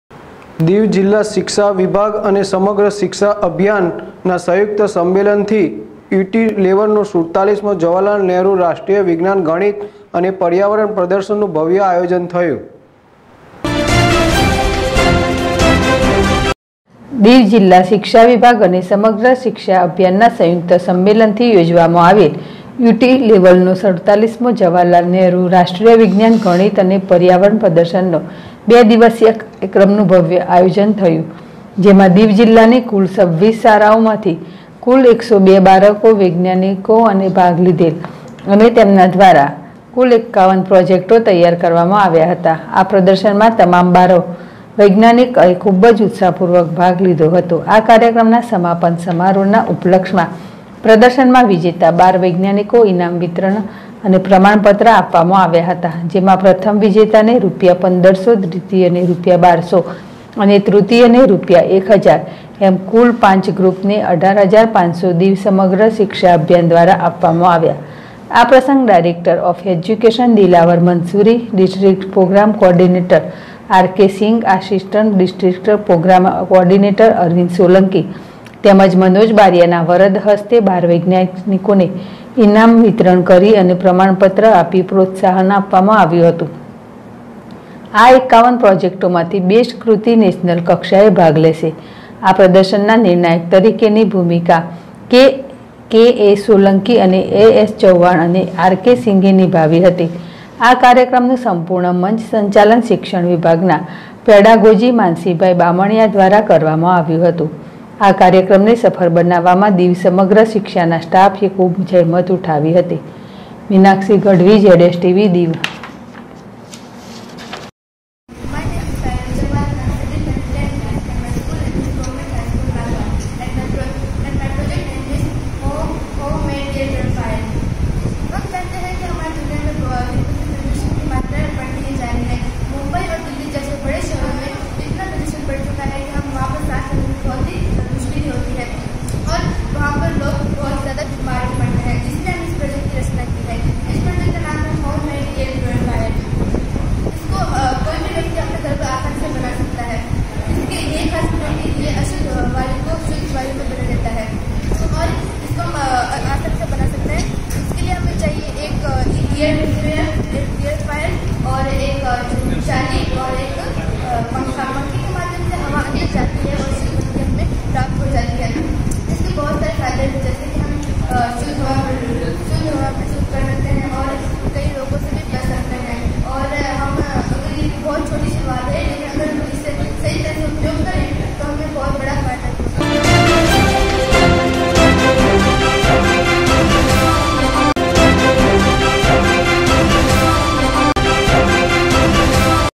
દીવ જીલ્લા શિક્ષા વિભાગ અને સમગ્ર શિક્ષા અભિયાન ના સંયુક્ત સંમેલન થી
દીવ જીલ્લા શિક્ષા વિભાગ અને સમગ્ર શિક્ષા અભિયાન ના સંયુક્ત સંમેલન થી U.T. લેવલ નો ૪૭ મો જવાહરલાલ નહેરુ રાષ્ટ્રીય વિજ્ઞાન ગણિત અને પર્યાવરણ પ્રદર્શન નુ ભવ્ય આયોજન થયું.